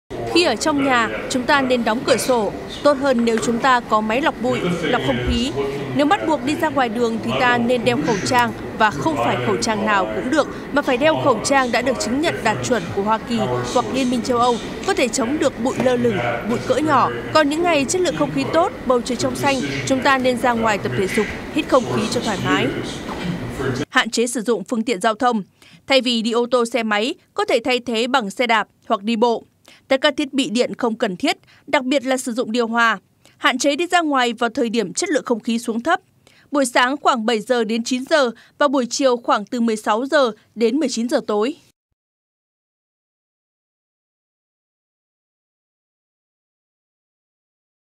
Boneco.vn | Chuyên gia Quốc Tế tư vấn cách bảo vệ sức khỏe khỏi ô nhiễm không khí tại Việt Nam
Tổ chức Y tế Thế giới (WHO) cho biết 9 trên 10 người trên thế giới hít thở bầu không khí ô nhiễm. Tình trạng này tước đi sinh mạng của hơn 6,1 triệu người mỗi năm, trong đó hơn 1,7 triệu nạn nhân là trẻ em dưới 5 tuổi.

Chuyên gia Quốc Tế tư vấn biện pháp giúp bảo vệ sức khỏe khỏi ô nhiễm không khí tại Việt Nam đặc biệt là tại 2 thành phố Hà Nội & Hồ Chí Minh
Tư vấn sức khỏe: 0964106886